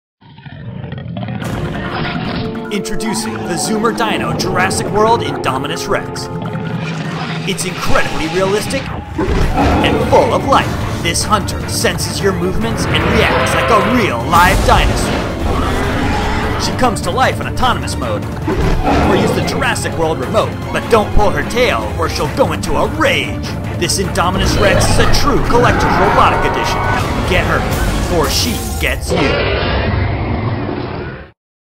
Zoomer Dino Indominus Rex Jurassic World od cobi.pl
Indominus Rex to nowy gatunek dinozaura stworzony przez genetyków Parku Jurajskiego. Ma przerażający wygląd i ma służyć jako nowa atrakcja Parku i przyciągnąć większą ilość spragnionych mocnych wrażeń turystów. Dinozaur wymyka się spod kontroli, co wprowadza chaos na wyspie… Podejmij wyzwanie, aby go oswoić w swoim domu!

Zoomer Dino Inodminus Rex wyposażony jest w 4 różne sposoby interakcji i zabawy. Tryb auto, tryb patrol, tryb ataku oraz tryb nagrywania, które zapewnią Ci wielogodzinną zabawę. Zachowuje się jak prawdziwy dinozaur: ryczy, macha ogonem, kłapie paszczą, porusza się. Każ mu patrolować teren, atakować lub nagraj sekwencję zachowań, aby zadziwić innych!  Możesz też sterować nim za pomocą ruchów dłoni lub pilotem. Wyposażony jest w innowacyjna technologię True Balance, dzięki której jego ruchy są bardzo płynne i realistyczne. Zoomer swoje nastroje, a także wejście w poszczególne tryby komunikuje zmieniającymi się kolorami oczu.

Zabawka zawiera akumulatora 1 X LiPo 3,7 V/450 MAH;
Wymagane 3 baterie AAA (nie załączono)